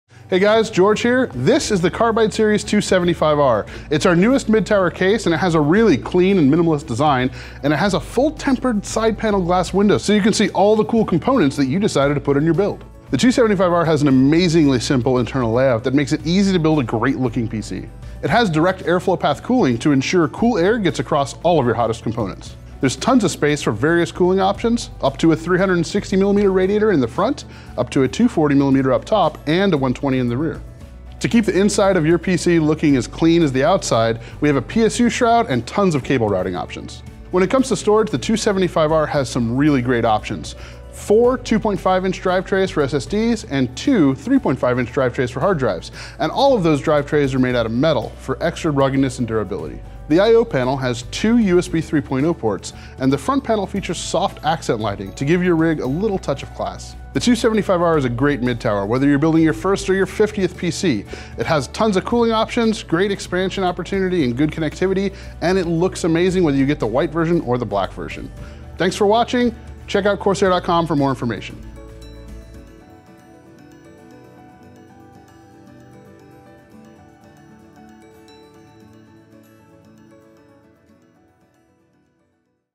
CORSAIR CARBIDE 275R - Product Breakdown
The CORSAIR Carbide Series 275R combines a sleek, minimalist design with all the features demanded by today’s most powerful PCs.

From edge to edge, the stunning tempered glass side panel shows off every inch of your build.